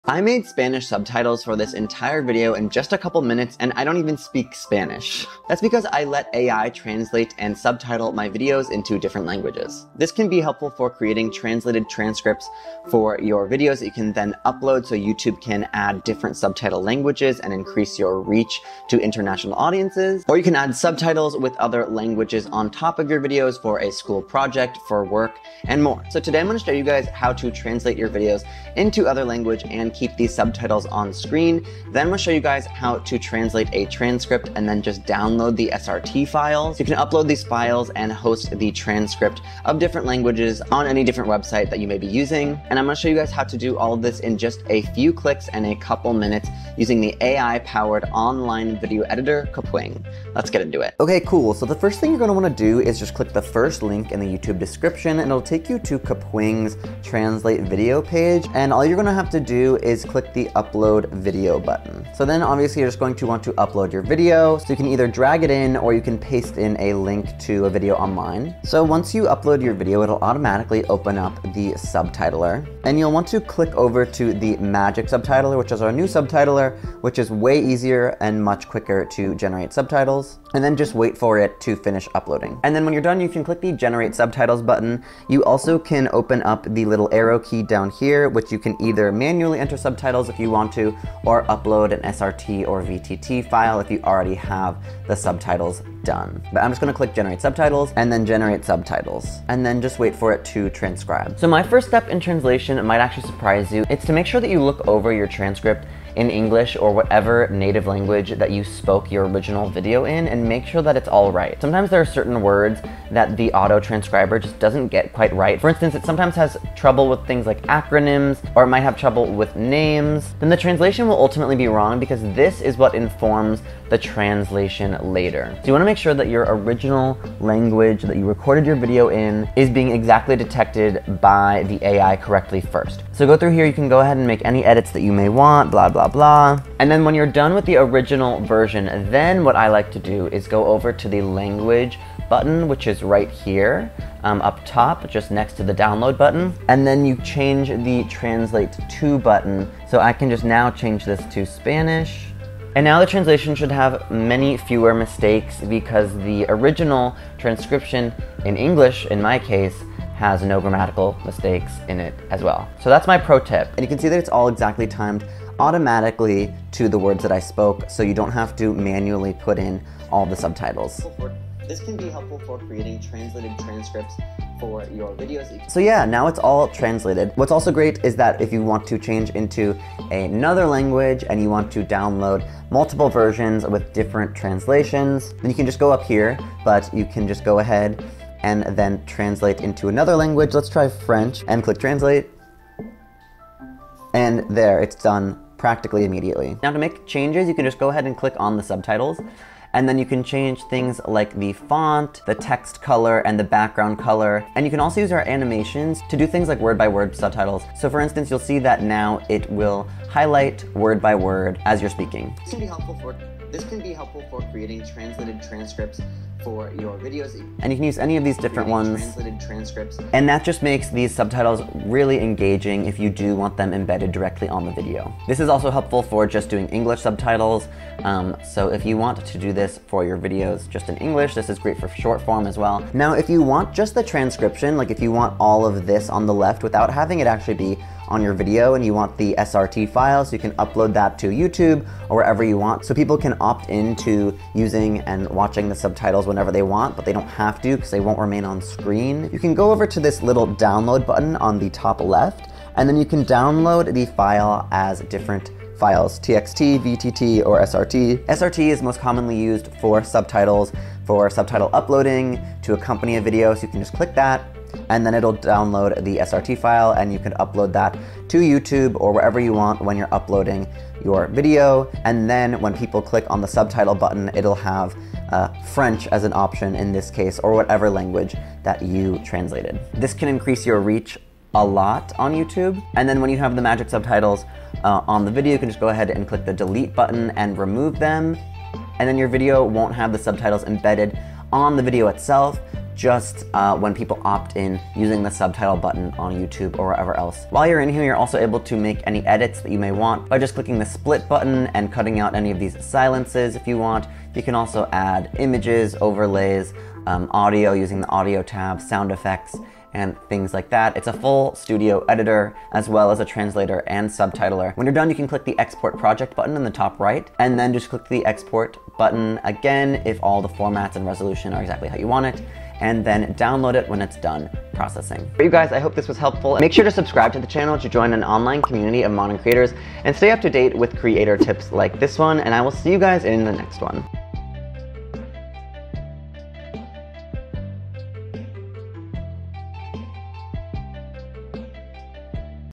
How to Use AI to Translate Your Videos into Different Languages
The power of AI has increased greatly, and now it can help you translate video and audio into other languages in just a couple minutes. All you have to do it upload a video or audio to Kapwing, the AI powered video editor,  and you can now make subtitles, SRT files, and animated captions to increase the engagement and international reach of your content.

The important of subtitling content and internationalization 0:00
Generating and editing a transcript in your native language 0:51
Turning your transcript into a translation 2:16
How to change the language of your transcript 2:57
How to make stylistic changes to subtitles and add animated subtitles  3:17
How to download the SRT files and get rid of on-screen captions 3:57
How to make edits to your video (split, audio, overlays) 4:57
Outro 5:26

(and yes, this is 2025 Amazing Race 37 Winner Jack!)